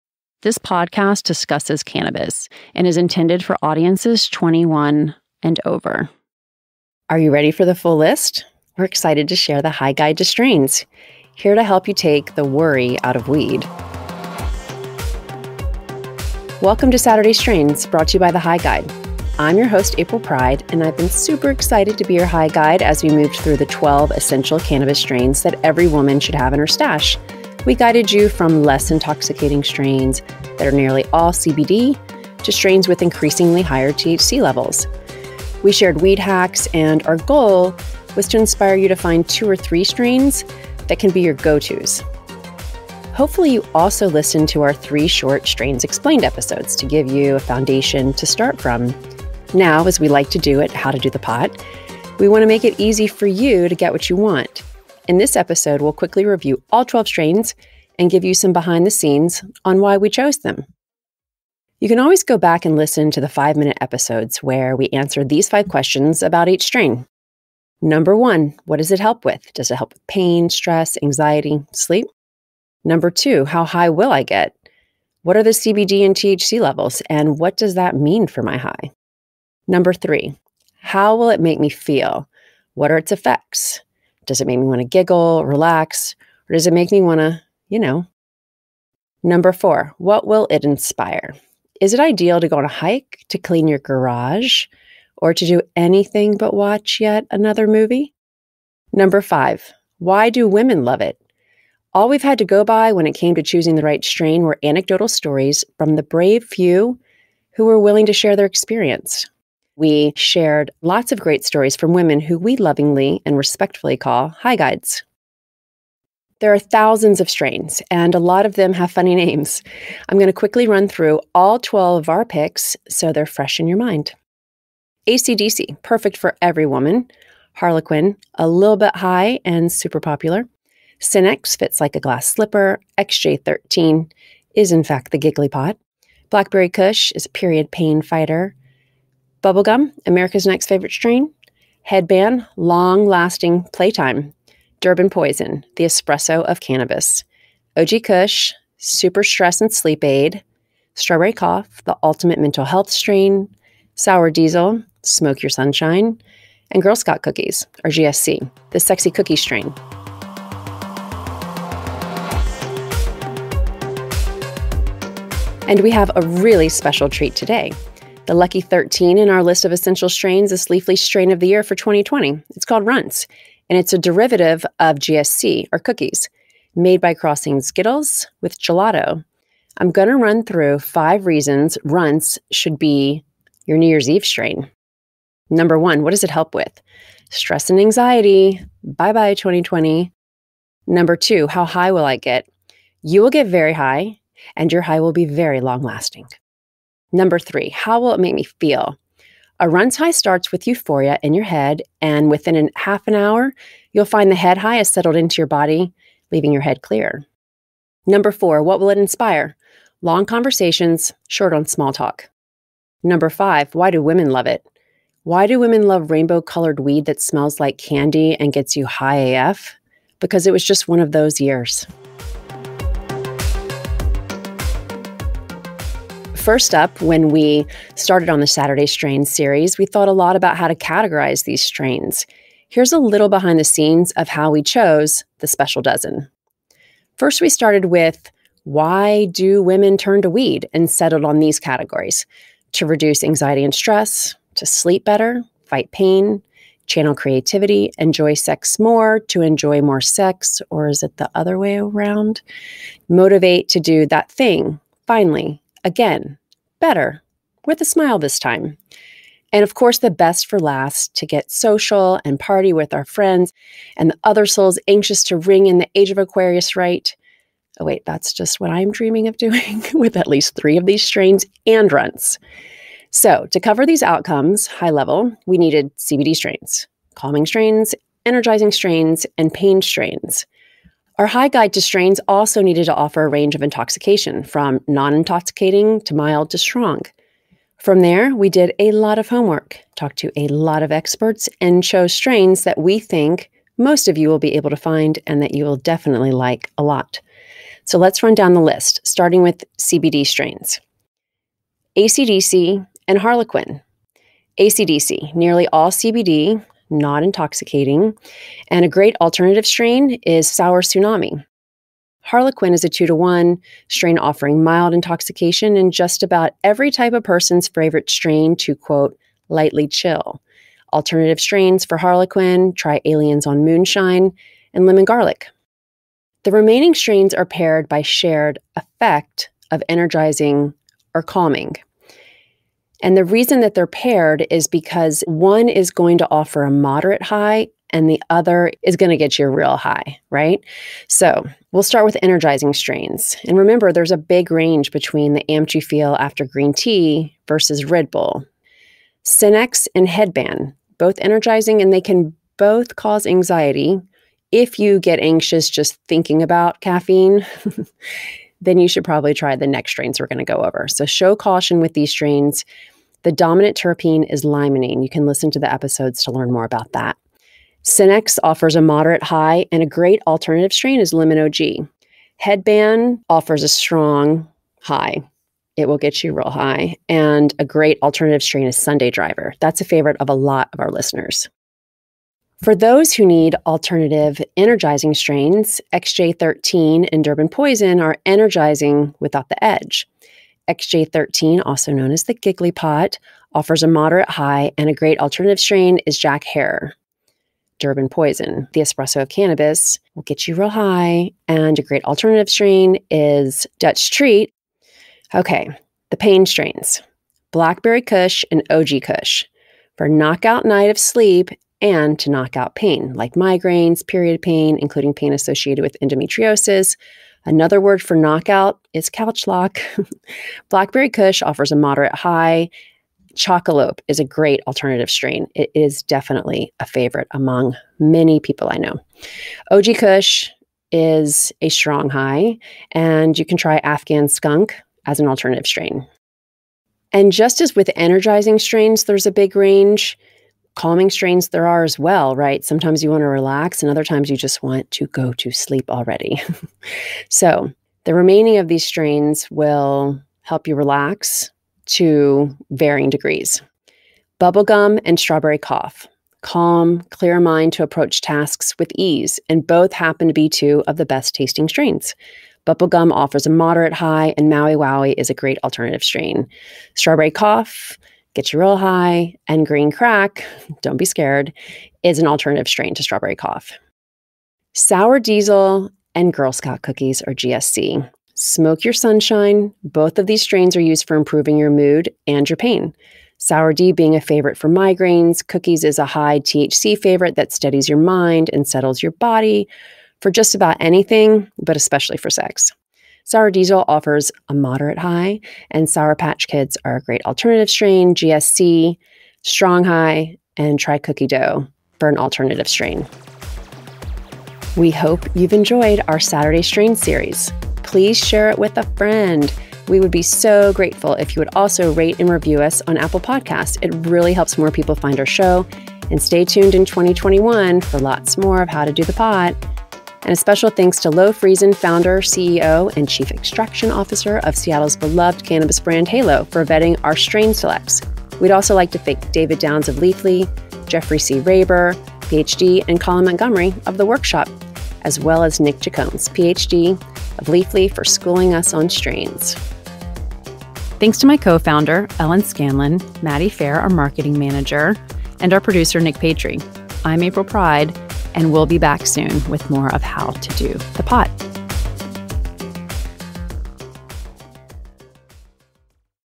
ESSENTIAL WEED STRAINS | Ep. 46 | HOW TO DO THE POT Podcast
Presenting The High Guide to Strains, the full Saturday Strains list. We run down all 12 of the essential cannabis strains that every woman should have in her stash, share behind the scenes secrets about how we chose the list, and offer alternative strains for each of our picks.

Looking for a New Year’s Eve strain? Check out our 5 reasons to try Leafly’s 2020 Strain of the Year, Runtz.

Saturday Strains is bringing the special back to Saturdays by demystifying the 12 essential cannabis strains that every woman should have in her stash.

For lots more tips on cannabis + women, visit The High Guide